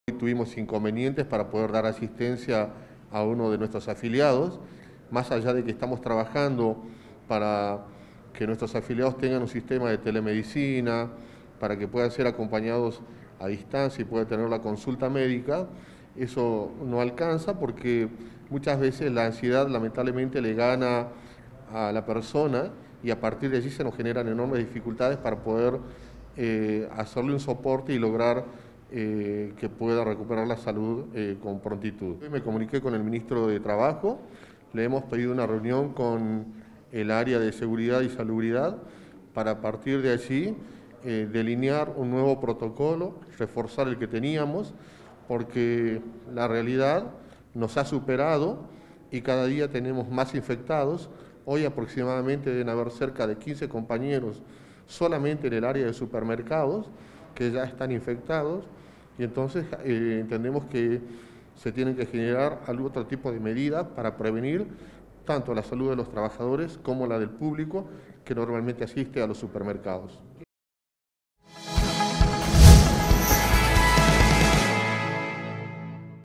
Almenos 15 empleados de comercio contrajeron COVID-19 | Somos Jujuy
Desde el Centro de Empleados de Comercio indicaron que la situación es dramática.